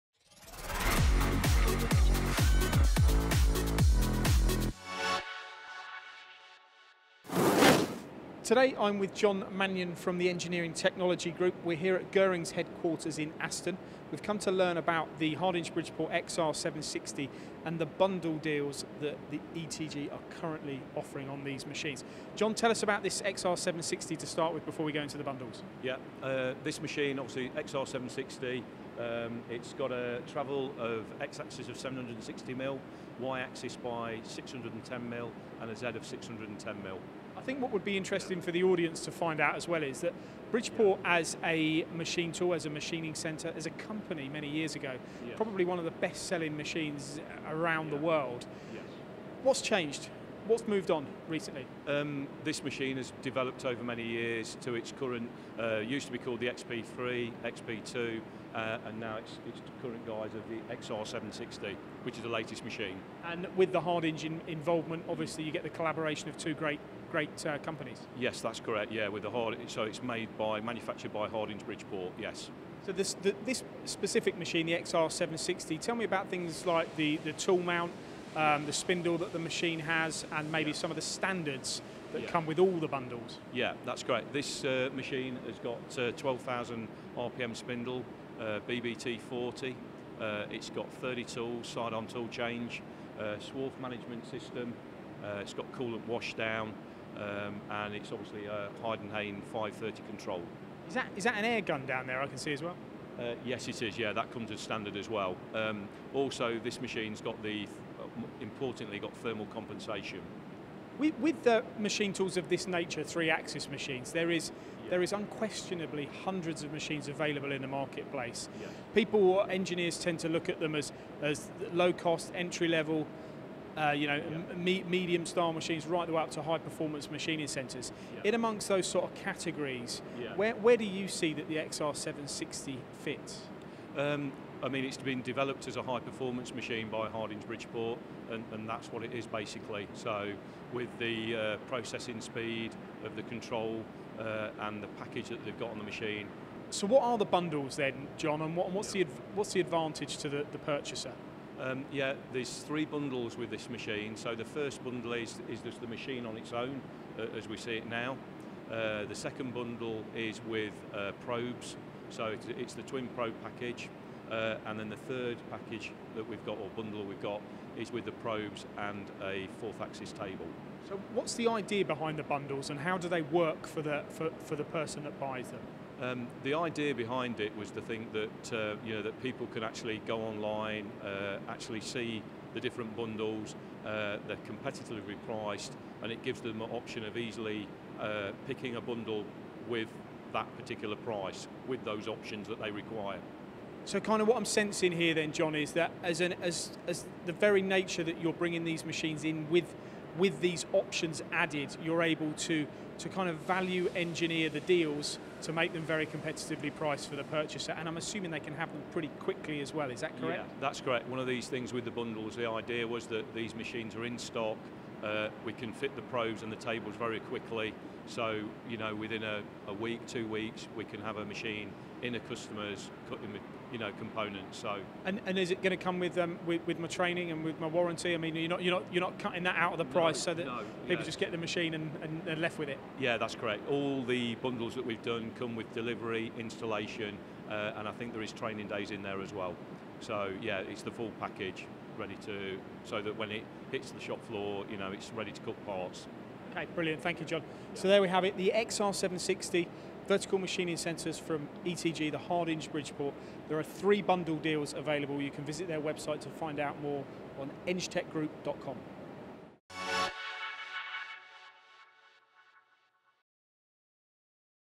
Hardinge Bridgeport XR760 Bundle deal from ETG
Every engineer likes a deal and the new Hardinge Bridgeport XR 760 is being offered as just that. Jon Mannion from ETG talks to Paul about this high performance model of machine tool We discuss the features that come as standard and the options included in the bundle deals.